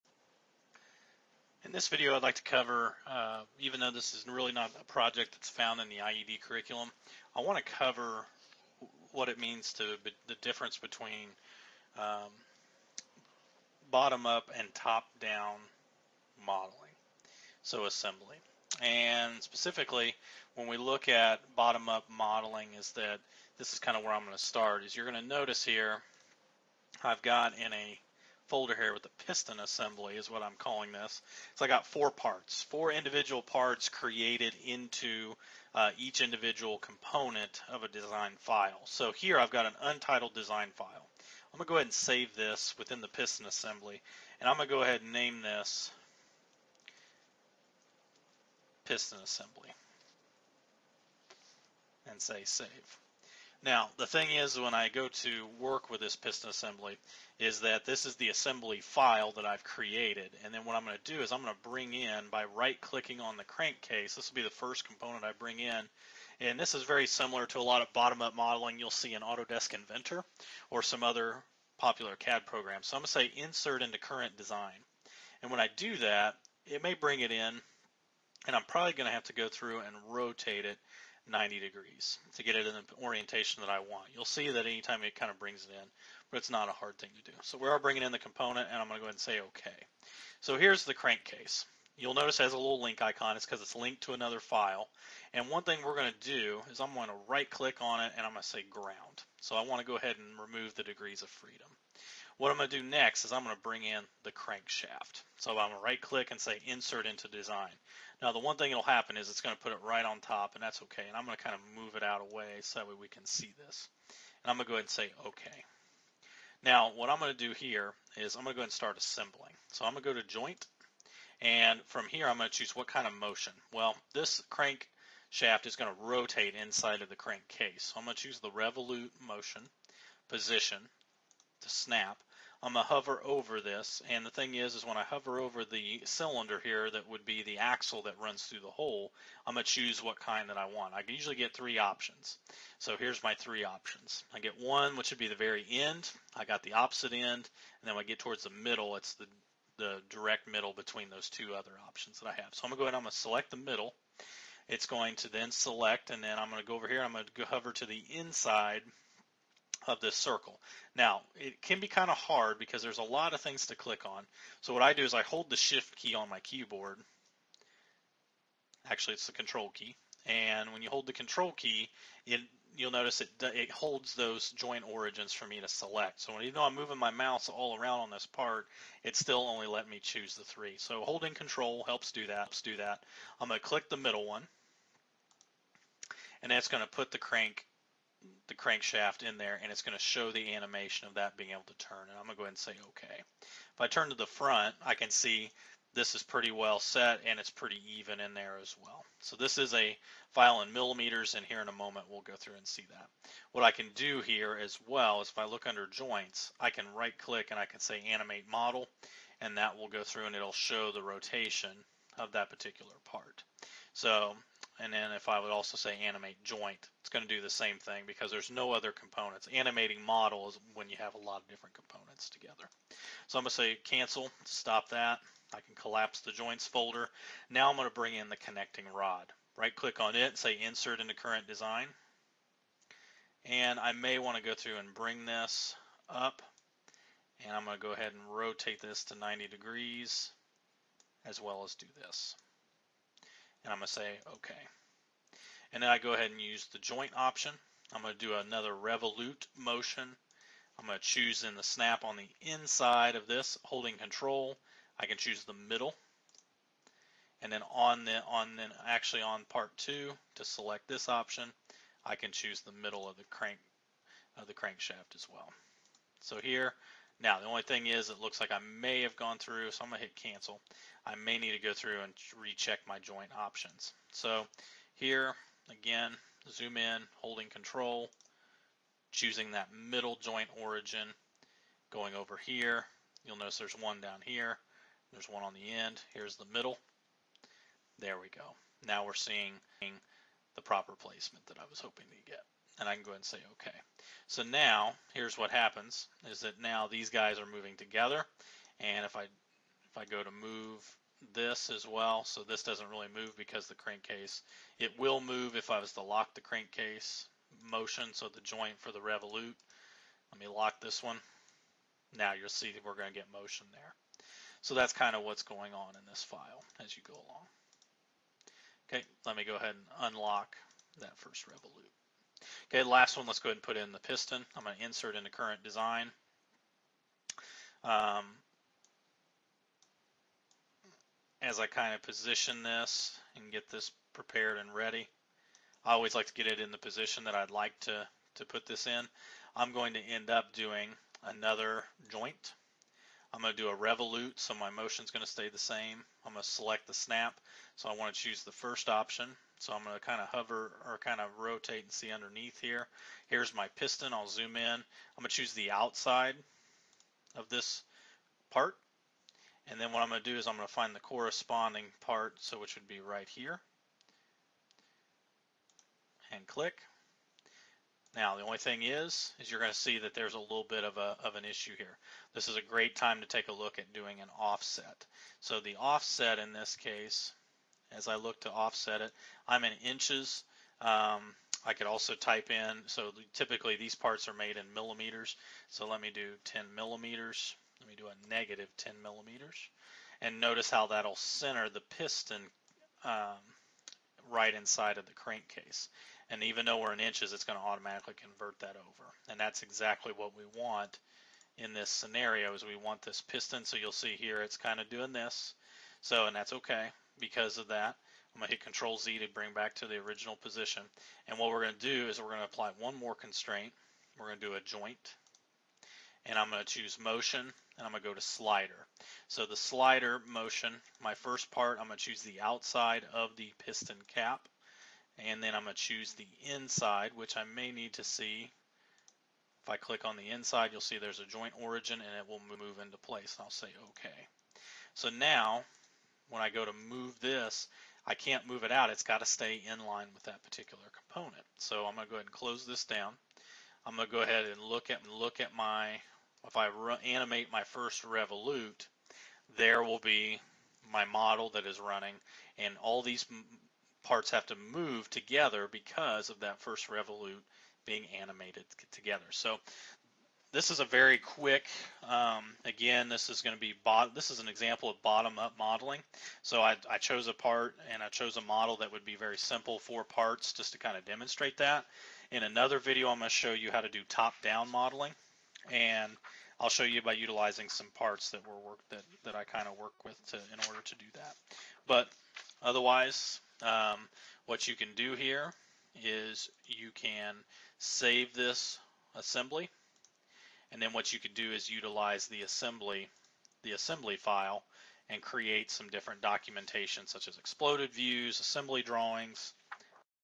Bottom Up Modeling in Fusion 360
If you wish to implement this modeling exercise into your classroom or just practice assembly in Fusion 360, use the link below to download the parts from the shared Google folder: